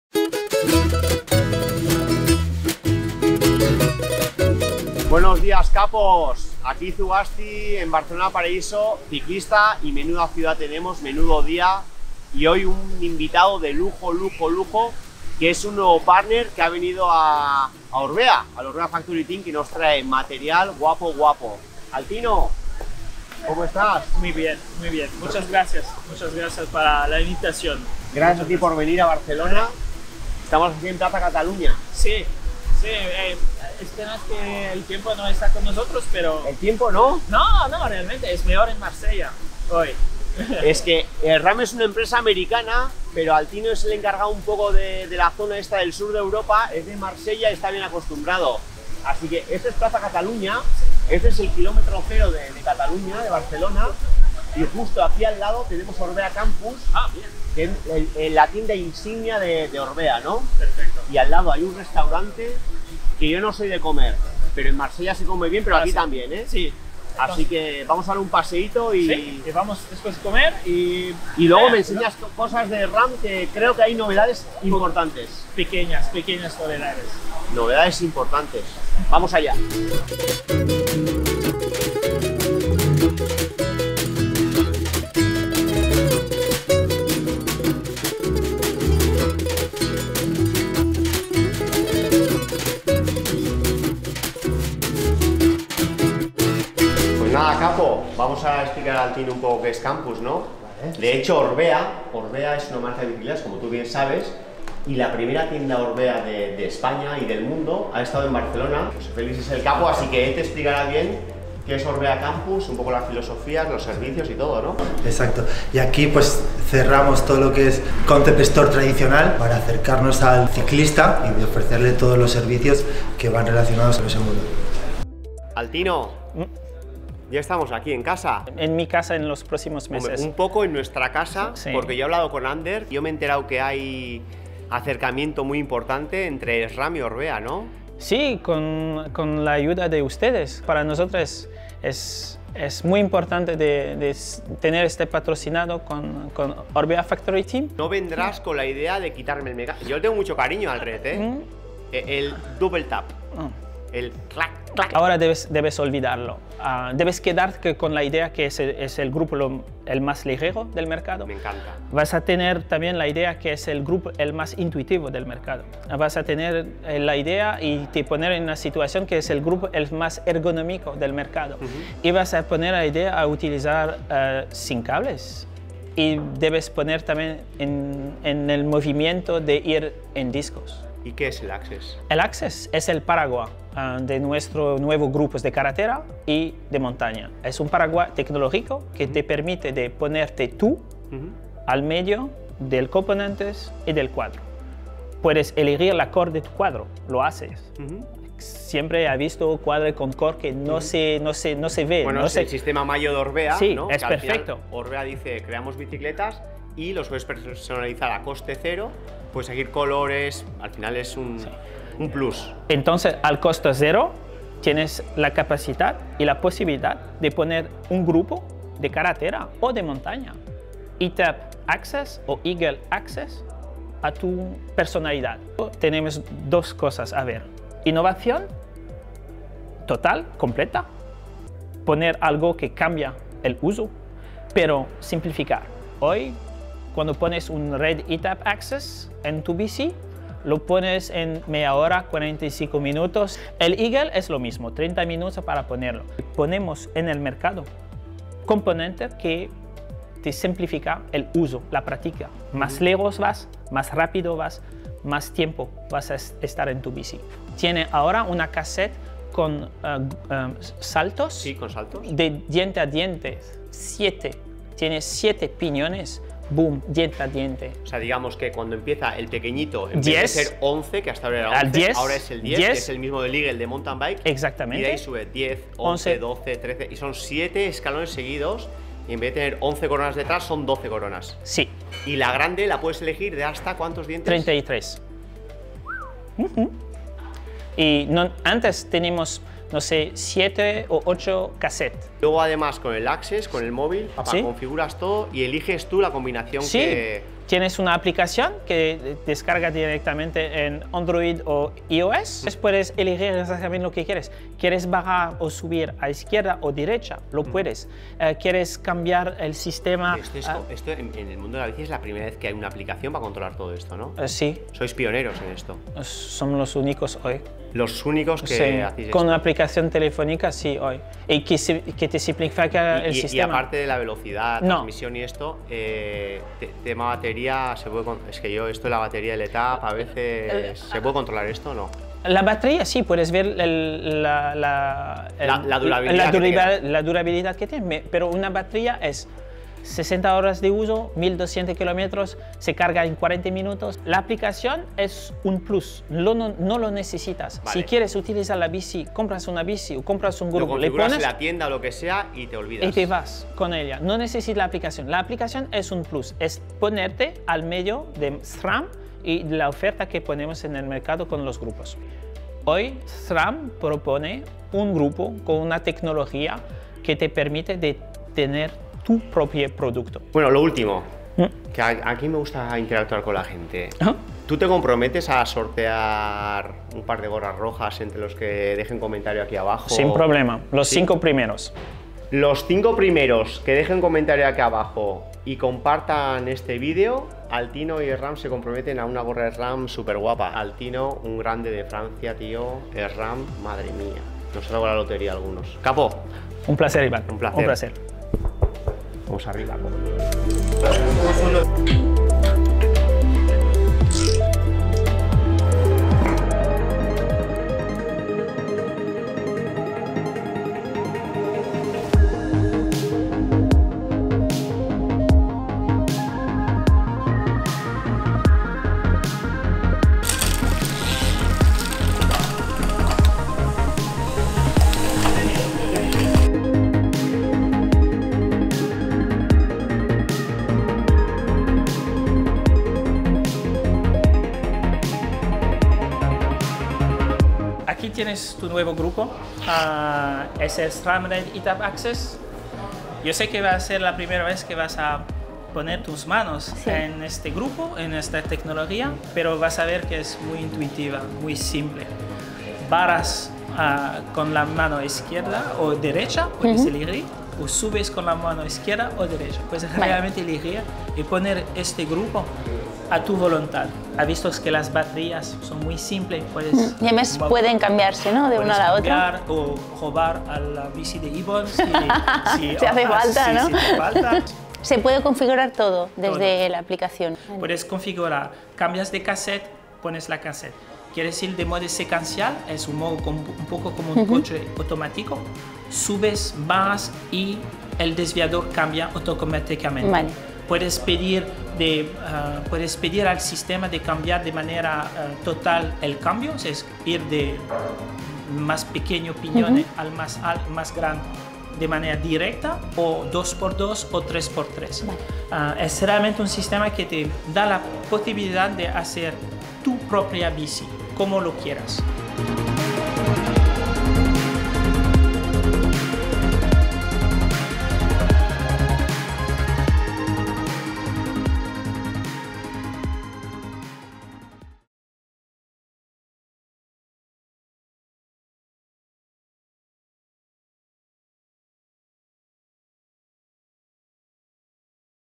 Tecnología SRAM AXS™ junto a Ibon Zugasti y Sandra Jordá I Orbea Factory Team
Nuestros riders del Orbea Factory Team, Ibon Zugasti y Sandra Jordà, reciben la visita del responsable de SRAM para el Sur de Europa, Altino Lourenco, para hablar del nuevo grupo SRAM Red eTap AXS™ y todo lo que hay tras la tecnología AXS™, tanto en el grupo de carretera como en el grupo para MTB, el SRAM Eagle AXS™.